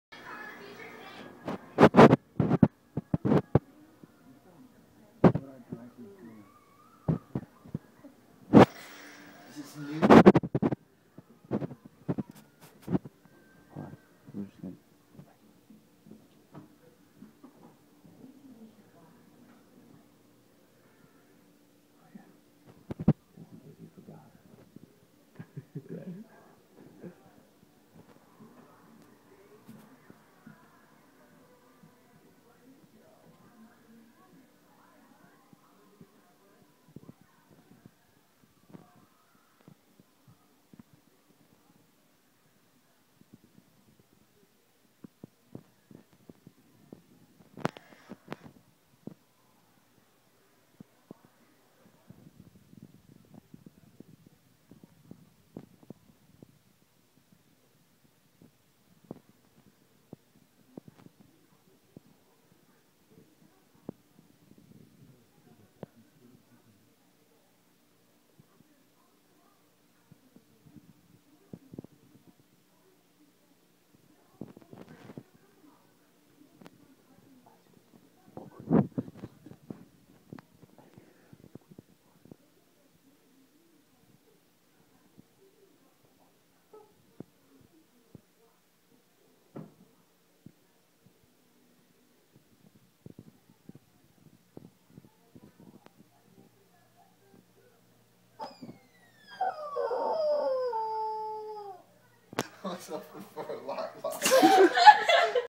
Day 4 - Madison County Film - Behind the Scenes
An example of why you shouldn't be the first to pass out. Not even our director is safe! Good thing he had his script for protection.